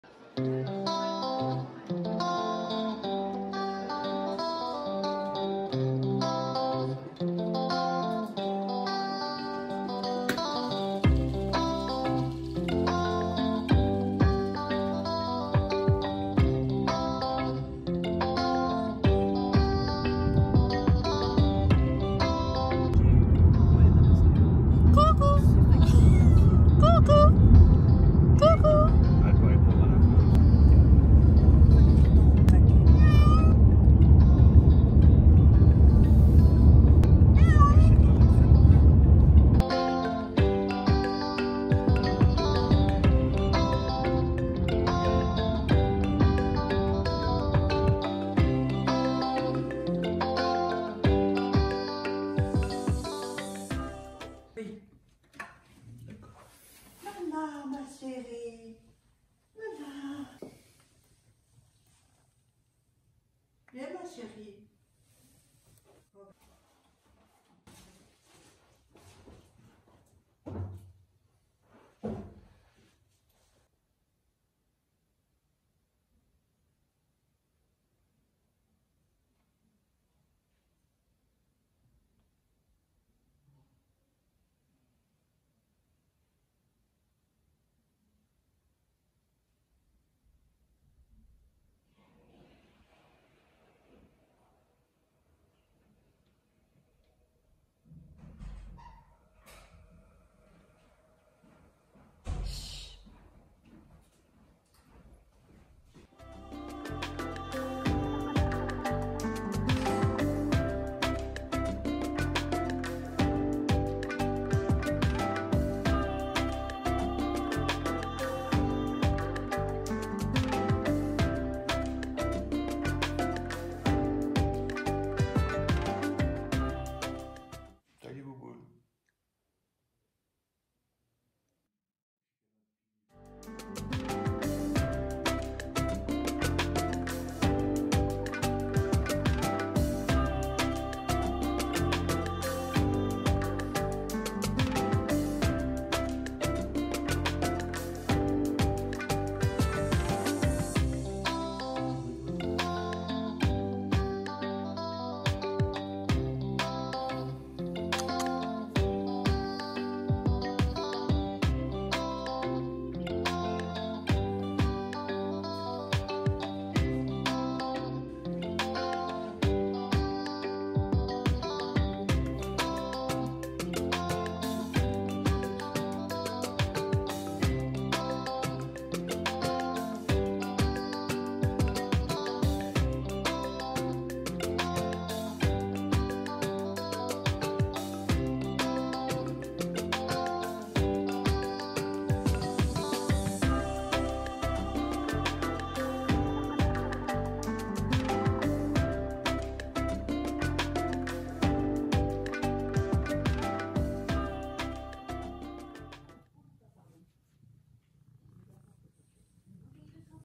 #07 [Sub] 새가족이 된 무스카델
집에 도착한 첫 이틀간의 영상입니다. 아직 많이 낯설어하지만 굉장히 순하고 사람을 좋아합니다. 너무나 사랑스러워요!